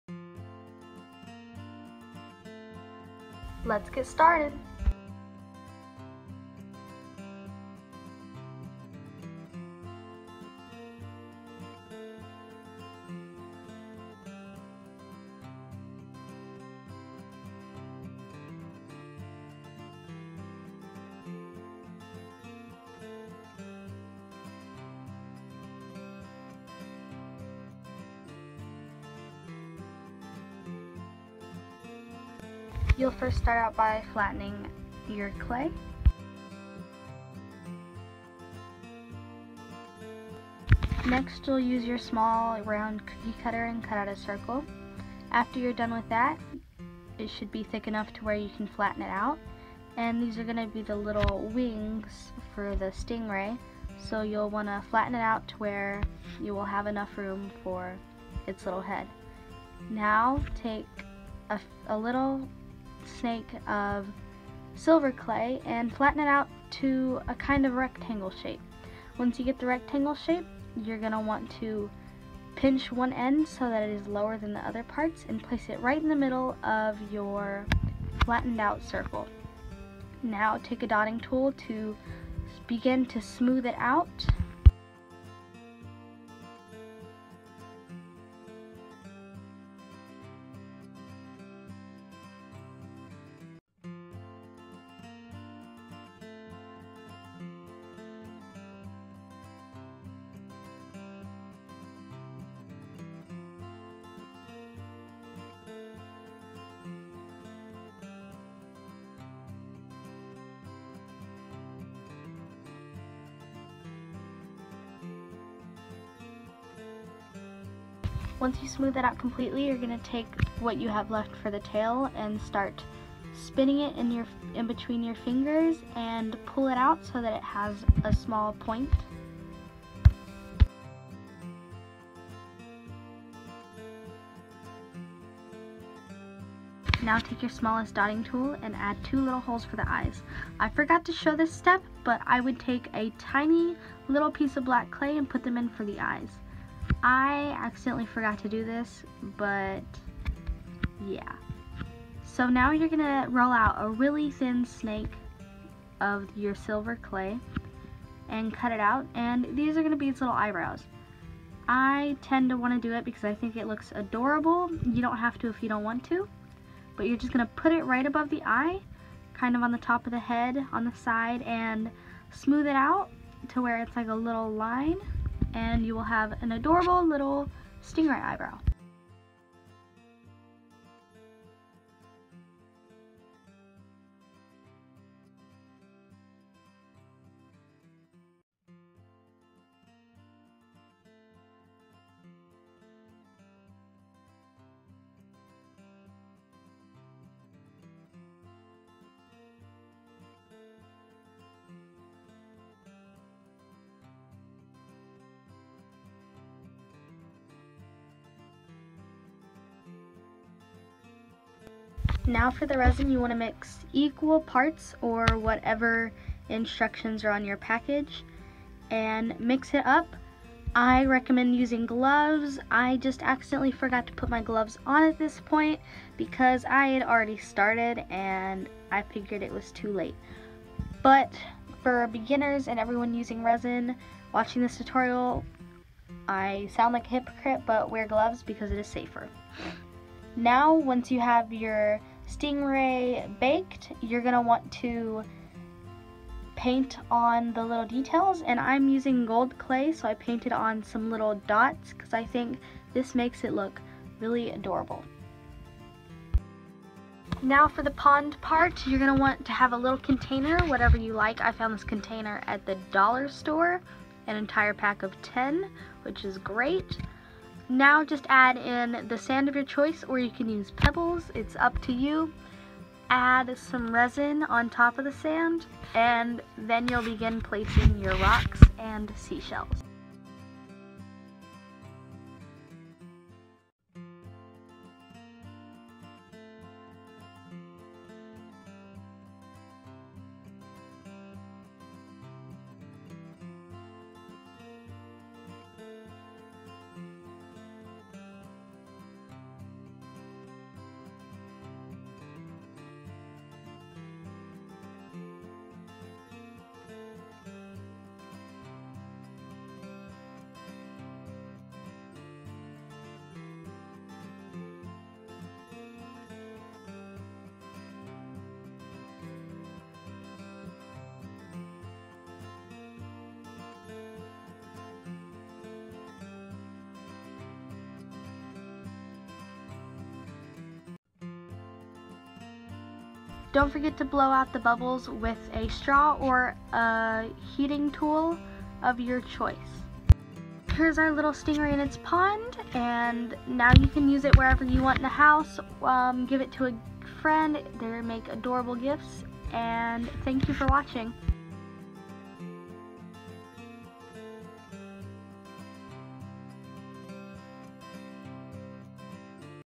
Polymer clay/Resin Stingray Pond tutorial
In this cute tutorial I'll be showing you how to make a resin and polymer clay stingray pond.

This is my very first tutorial here on YouTube

It would help me so much.

Also☺️

Check out my etsy where I sell many of my Resin charms. You're support would mean the world.